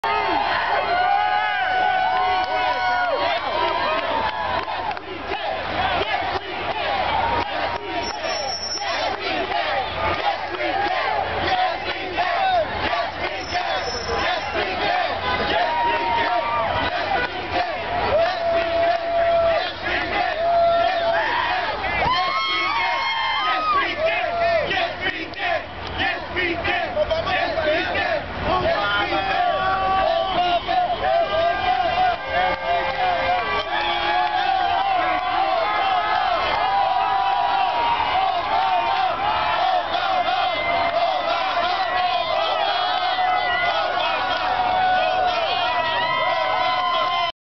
Revelers chant "Yes, We Did!" in Times Square at Obama win
As ABC's camera cranes above them, people of every walk of life chant, "Yes, We Did!" minutes after Obama spoke.